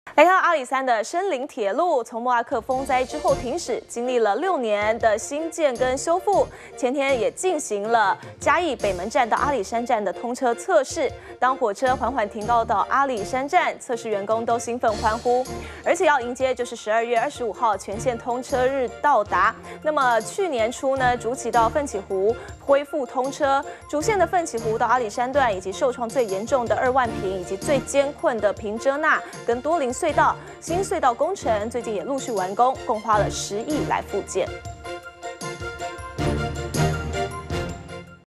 【2015.09.17】等了六年 阿里山森鐵上山測試 -udn tv
阿里山森林鐵路自莫拉克風災後停駛，歷經六年多重建與修復，前天進行嘉義北門站至阿里山站通車測試，當火車緩緩停靠阿里山站，測試員工興奮歡呼。迎接十二月廿五日全線通車到來。去年初竹崎至奮起湖段恢復通車，主線的奮起湖到阿里山段，及受創最嚴重的二萬平及最艱困的「屏遮那」和「多林隧道」新隧道工程，最近陸續完工，共花了十億復建。完整內容 , FB , Twitter , G+